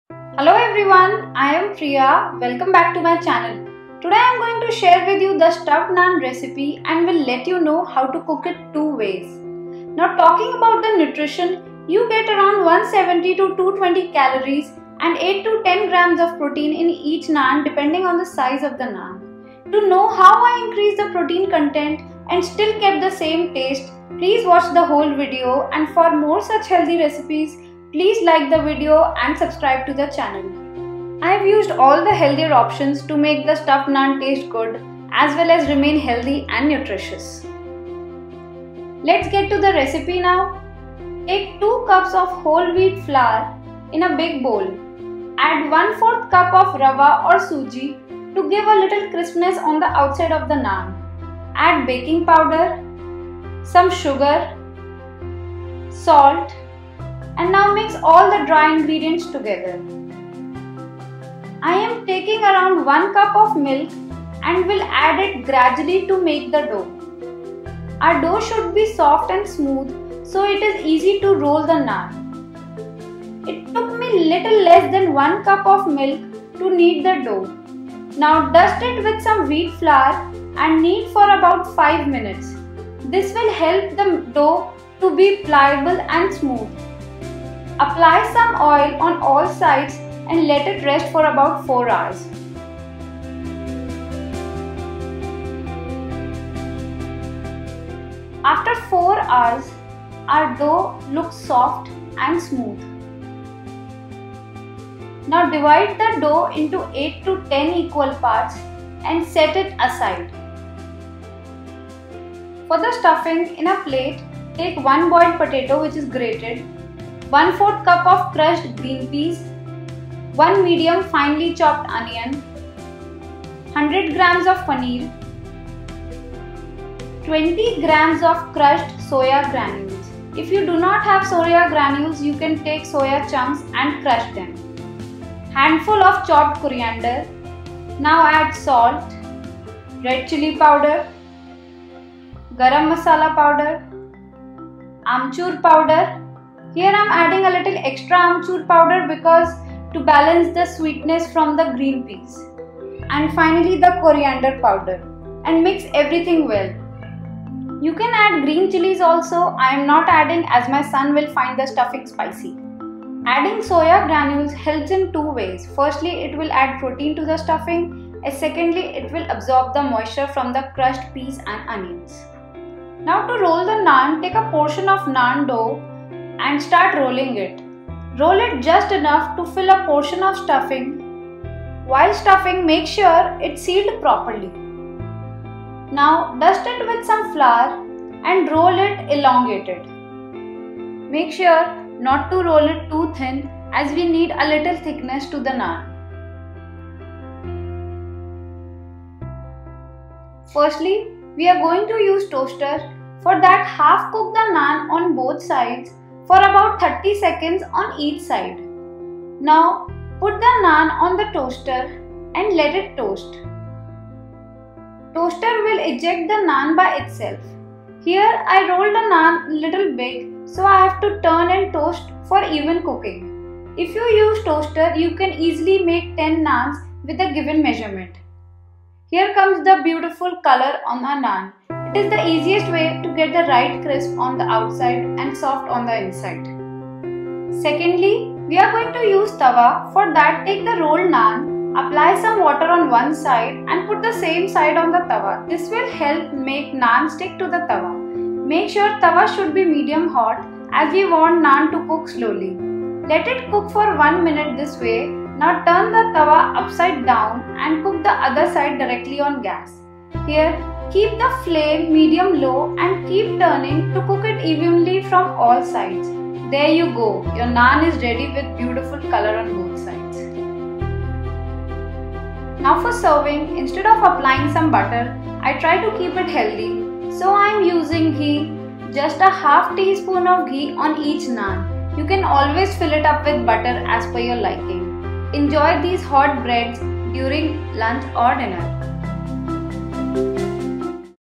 Healthy Stuffed Naan Recipe| भरवां नान तवा पर | Naan in the Toaster| No Oven Naan
Stuffed Naan is one of the most likable hot bread served in India. It is generally made with all purpose flour and stuffed with onion potato mixture.

To make it healthy, I have used Wheat Flour to make the dough and has added many protein rich ingredients in the stuffing. Enjoy this this hot bread during lunch or dinner without guilt.